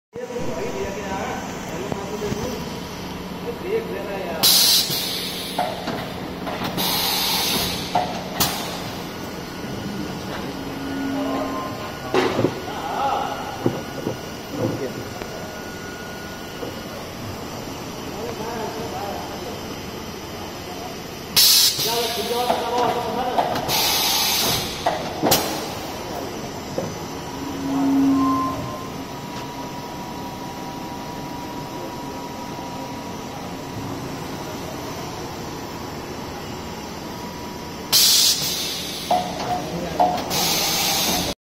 HARDING BRIDGEPORT VMC 710P3 CNC VERTICAL MACHINE CENTER
MAKE-Harding Bridgeport VMC-710 p3
Control Fanuc-oi mc
Table size 800x400
Travel size-710x400x430
RPM-10000
Bt-40
5.5 / 7.5 kW
Year 2010